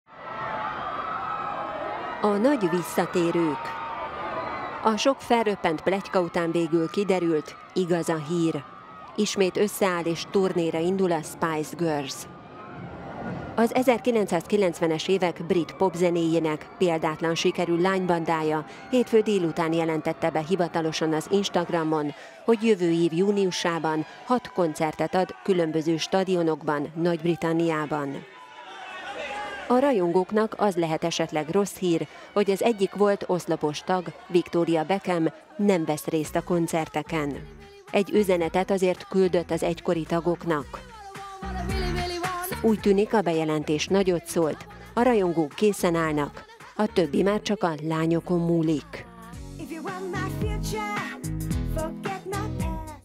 Újra összeáll a Spice Girls - Victoria nélkül
A példátlan sikerű brit lánybanda bejelentette, hogy jövő év júniusában hat koncertet ad különböző stadionokban Nagy-Britanniában.…